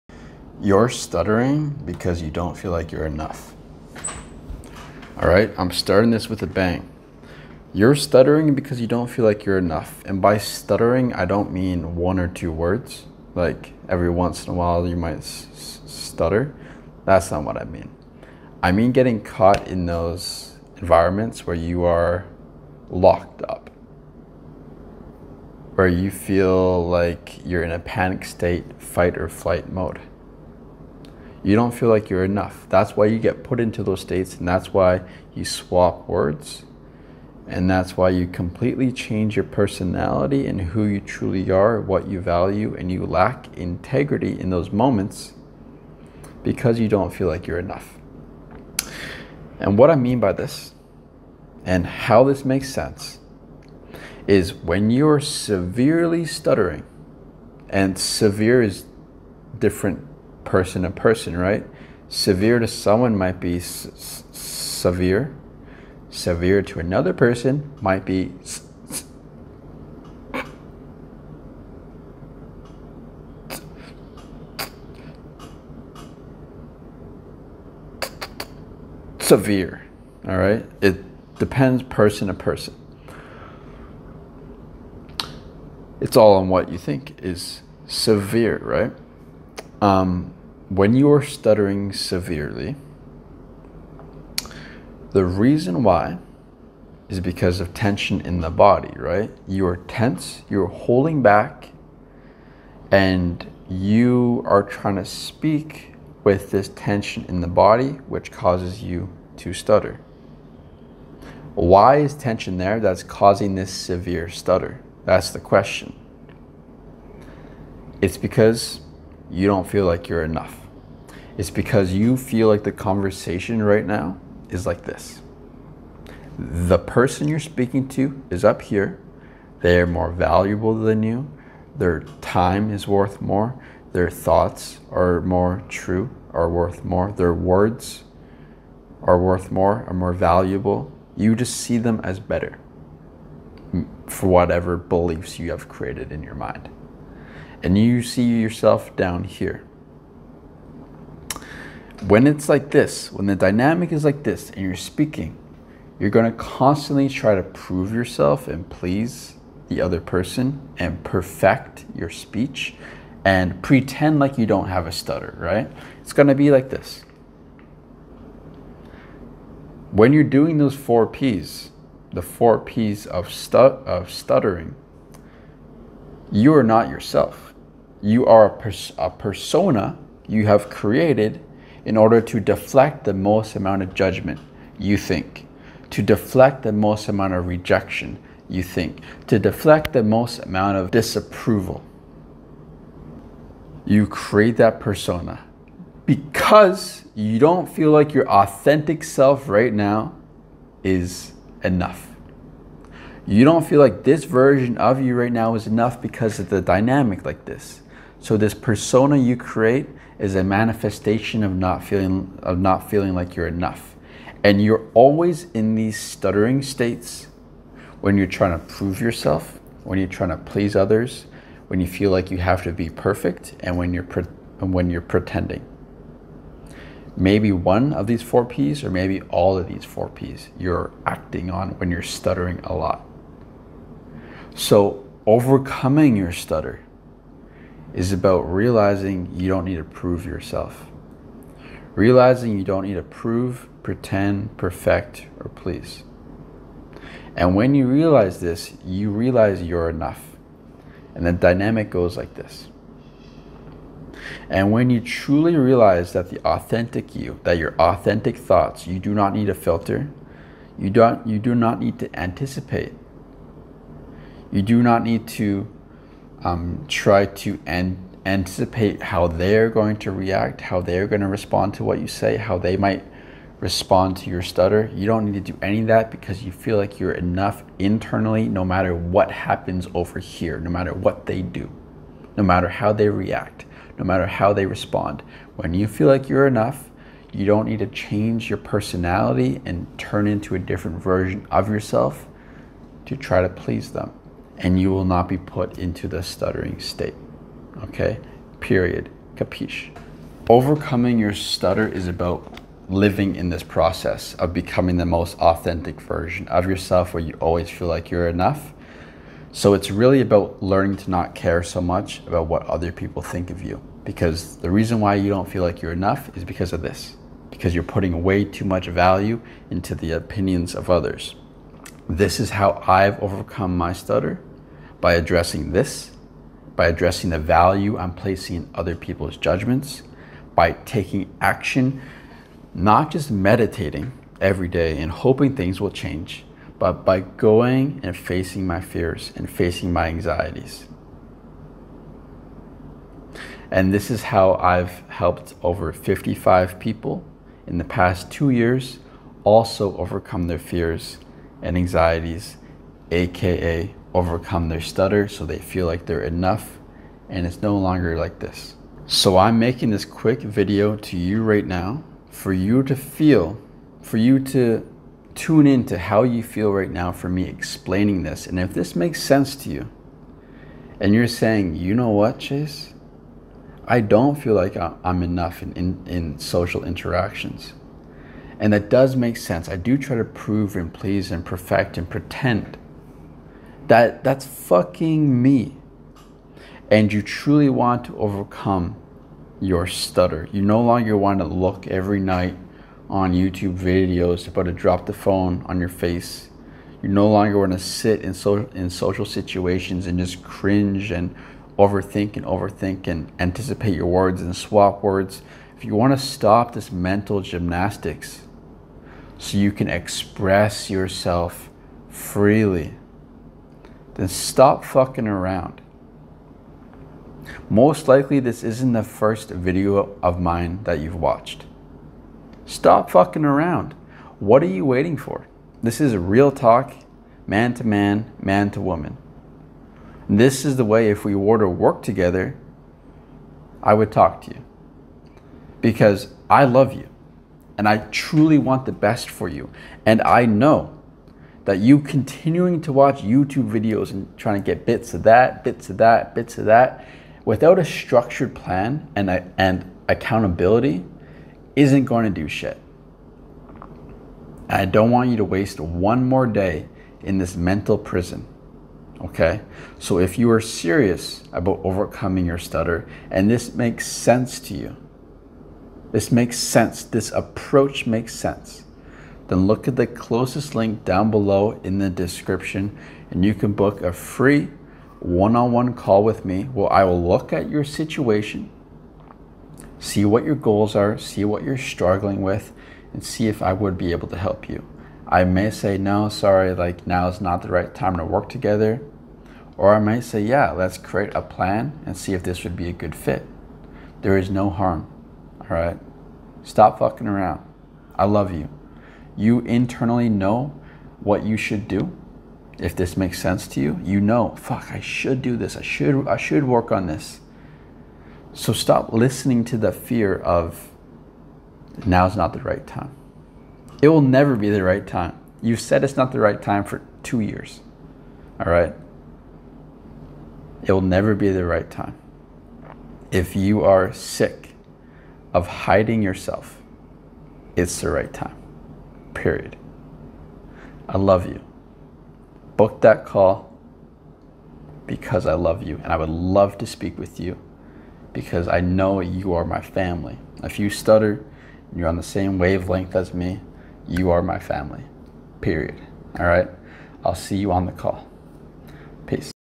You're Stuttering Because You Don't Feel Like You're Enough
I get this question a lot.. “Chase, what techniques do you use to overcome your stutter?”
The truth is, I would NEVER give you a “speech fluency technique” for you to stop stuttering. WHY? Because these techniques never address the root problem of why you are ACTUALLY stuttering. 99% of the time these techniques only serve as a crutch because your relationship with stuttering is so negative you will do anything to cover it up.

So how can I help you? I can help you if you are ready so forgive techniques and are ready to dive into the new world of authenticity. All your life you have been hiding. Hiding your true personality, hiding from environments, hiding from judgements, hiding from social situations. Overcoming stuttering is about no longer hiding. No longer holding back. Expressing your TRUE AUTHENTIC self that you have been starving the world and yourself from enjoying. When we work together I lead you down the system that has gotten myself and my clients from being scared and isolated to being fearless and authentic.

The world needs to see the real you. You have so much to give. You are funny. You are talented. You are articulate. You are expressive. You are charming. You are attractive. Let's uncover all that is blocking yourself from becoming that person you know you are.